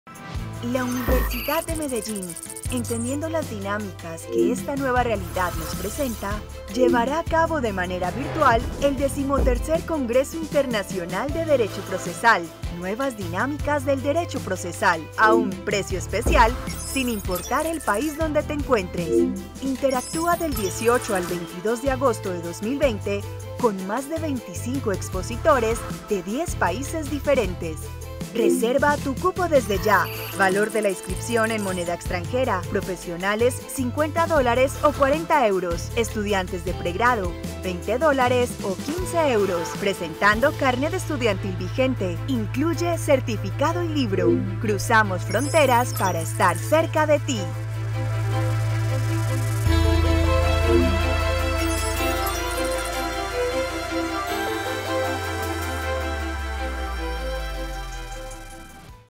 XIII Congreso Internacional de Derecho Procesal: Valores Internacionales
La UdeMedellín presenta la versión XIII del Congreso Internacional de Derecho Procesal en el cual se abordaraán temas como: Objetivos y Desarrollo Sostenible, Mecanismos Alternativos de Solución de Conflictos, Cuarta Revolución Industrial y Habeas Data Ambiental.
¡No te lo podes  perder!